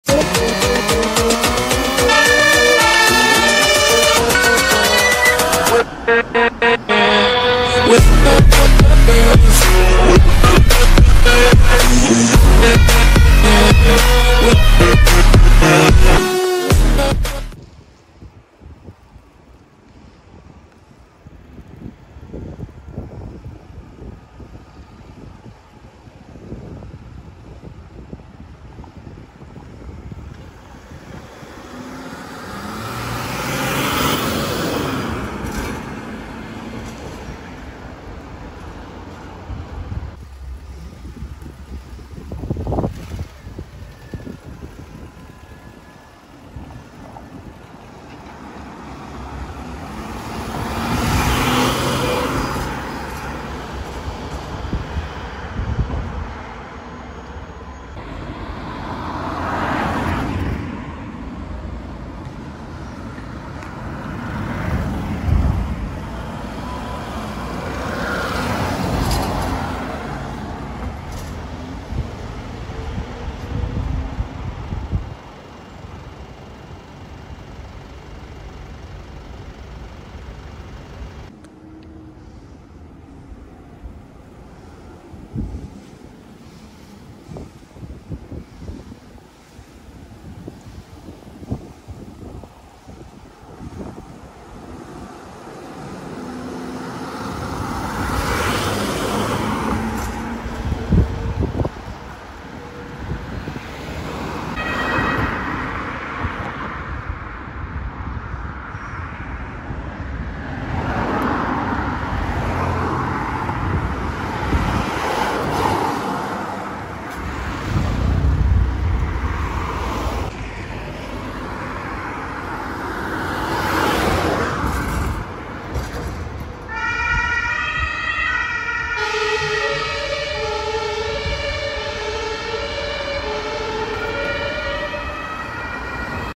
RTW Zusammenschnitt von der FRW 1
Zusehen ist ein kleiner Zusammenschnitt was noch so gefilmt wurde an RD vor der FRW 1 Hannover.